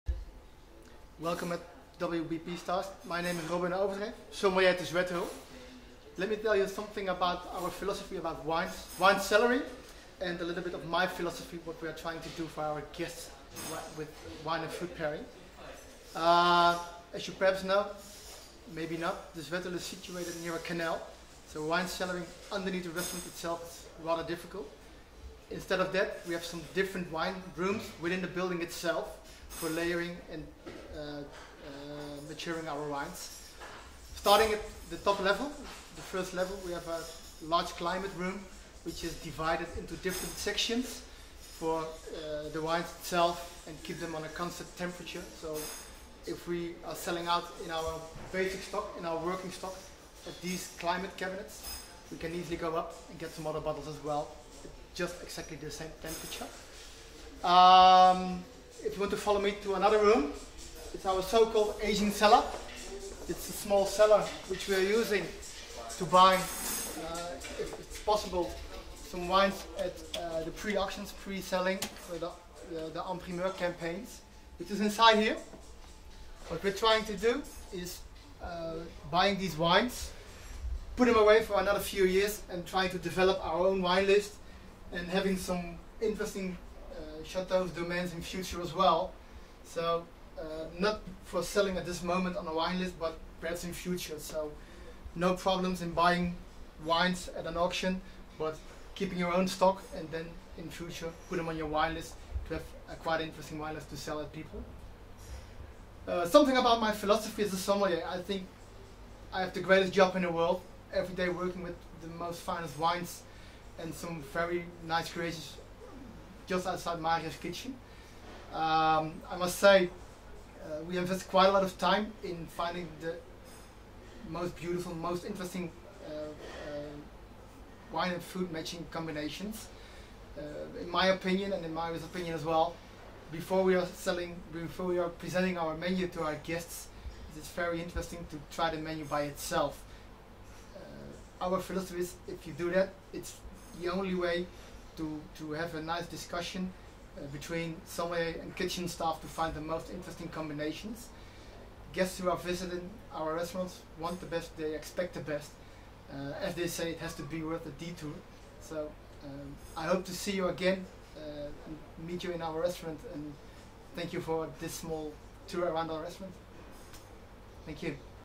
Robin Overdreef gives a tour of the wine cellar at 2 Michelin De Zwethheul
Head Sommelier Robin Overdreef gives a tour of one of the wine cellars at the 2 Michelin star restaurant De Zwethheul in The Netherlands.